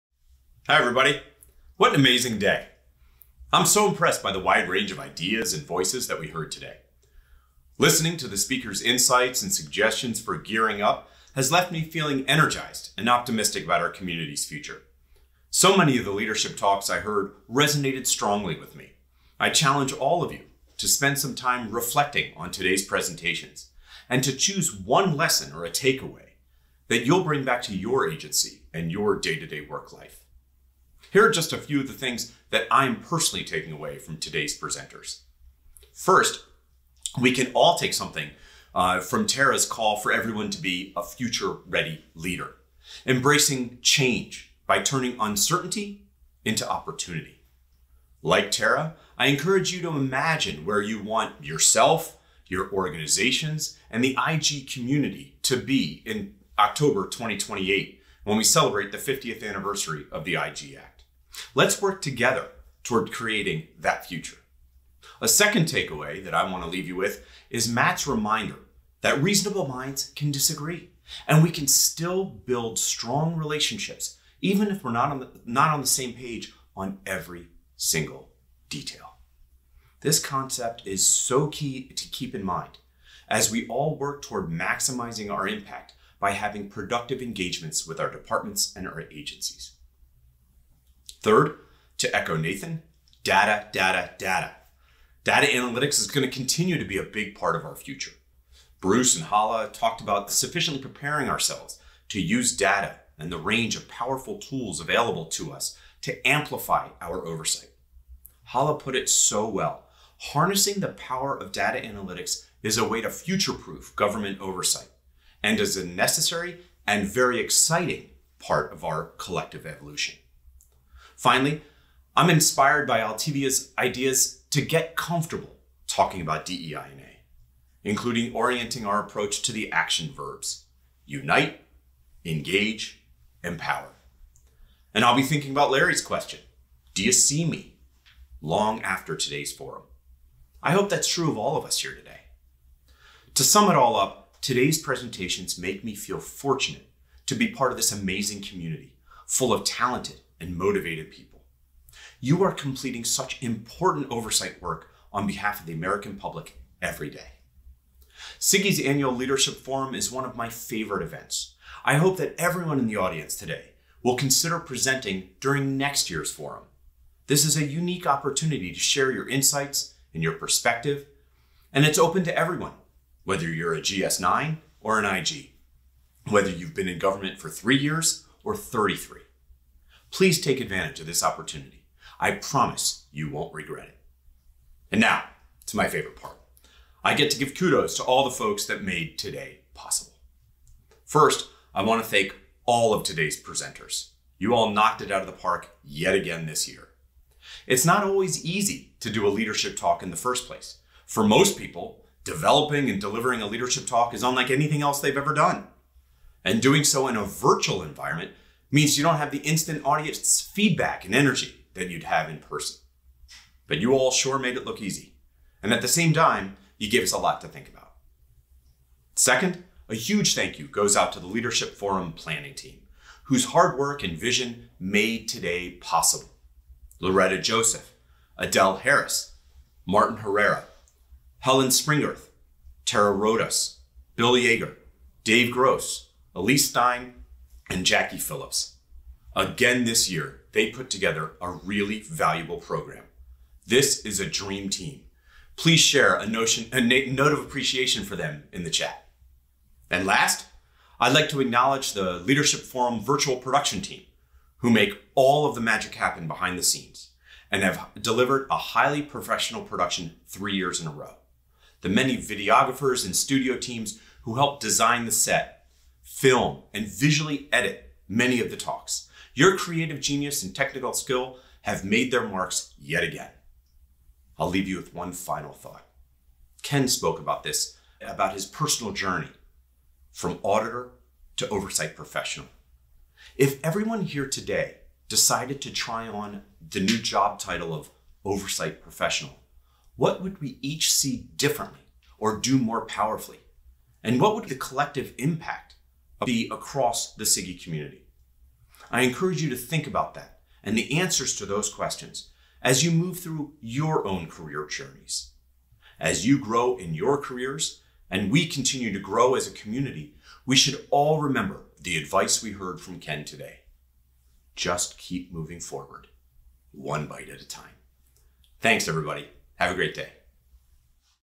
Leadership Forum Closing: The Honorable Mark Lee Greenblatt
The Honorable Mark Lee Greenblatt
Inspector General
U.S. Department of the Interior & CIGIE Vice-Chair